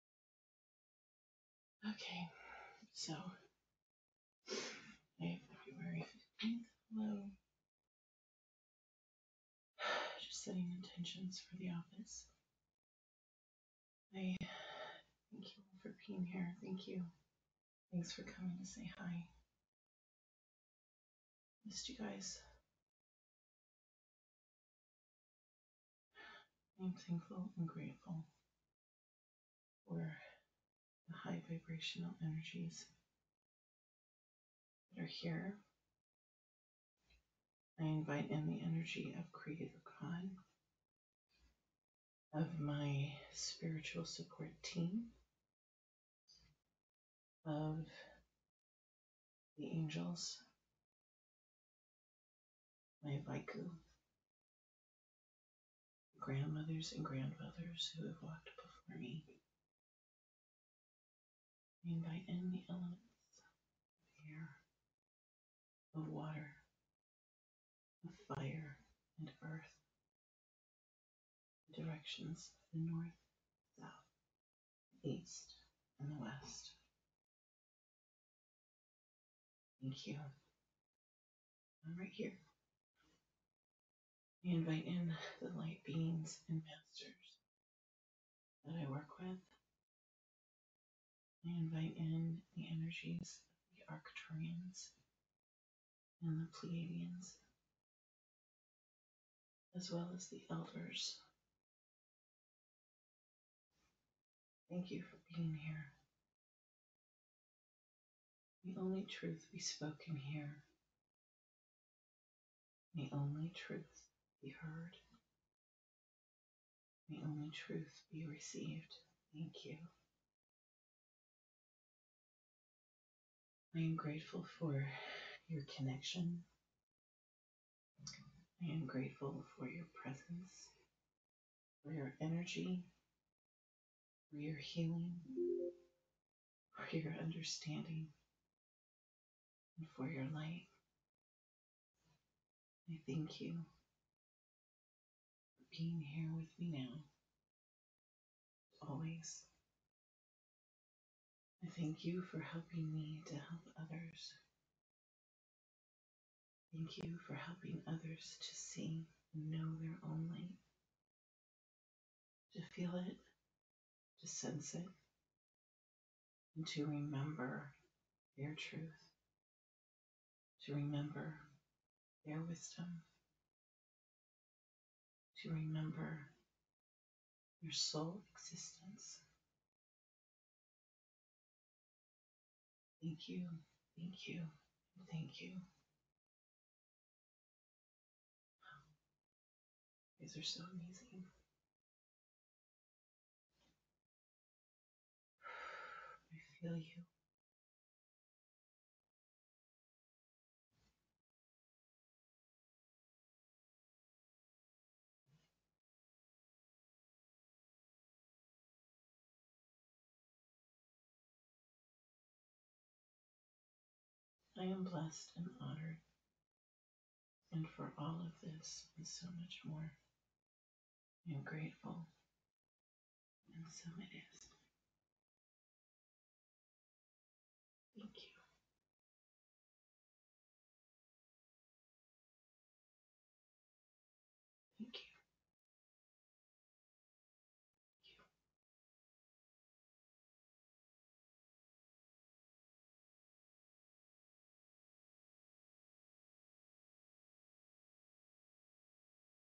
Another Awesome Day in My Office
There's an open portal on the wall at the head of the massage table.  You can see the energies coming through it.

When I come in to the office, I connect with the energies of my guides and angels and set intention.  I love how they respond!!!

Such an awesome and amazing experience.  I am blessed.

So, check out the orb show.  Can you see the differences in light, size, shape, and speed??